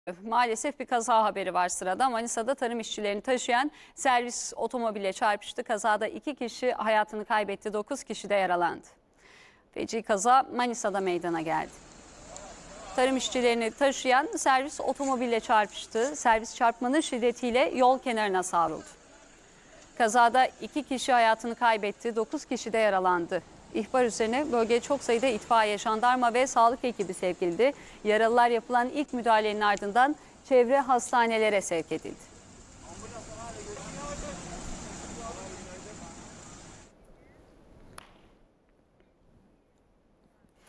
Manisa'da işçi servisiyle otomobil çarpıştı: 2 ölü, 9 yaralı | A Haber
Kaza, sabah 07.30 sıralarında Manisa'nın Alaşehir ilçesi Kula yolu Tepeköy kavşağında meydana geldi. Edinilen bilgiye göre Kula'dan Alaşehir'e işçi getiren Bekir Karabulut (50) idaresindeki minibüs, Alaşehir'den Salihli yönüne giden Gülser Akkaya (36) yönetimindeki otomobille çarpıştı.

Kazayı gören çevredekilerin haber vermesi üzerine olay yerine çok sayıda 112 acil servis ekibiyle Manisa Büyükşehir Belediyesine bağlı itfaiye ve jandarma ekipleri sevk edildi. Sağlık ekiplerince yapılan incelemede otomobil içerisinde bulunan sürücü Gülser Akkaya'nın kayınvalidesi Neriman Akkaya (73) ile otomobil içerisinde bulunan ve sürücü Bekir Karabulut'un eşi Emine Karabulut (48) olay yerinde hayatlarını kaybetti.

Öte yandan kaza nedeniyle her iki araçtan da toplamda 9 kişi yaralandı. Yaralılar ambulansla çeşitli hastanelere kaldırılarak tedavi altına alındı.

Kazayla ilgili soruşturma başlatıldı.